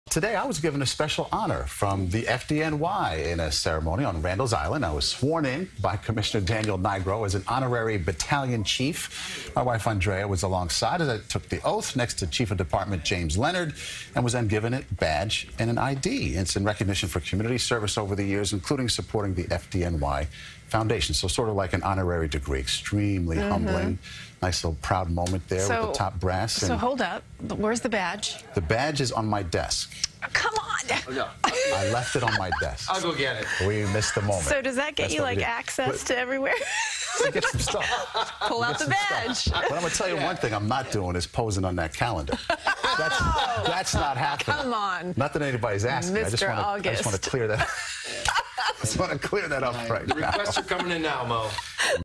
Maurice DuBois Named FDNY Honorary Battalion Chief
CBS2 News co-anchor Maurice DuBois was honored in a ceremony on Randall's Island on Thursday with an honorary battalion appointment and badge in recognition for community service over the years.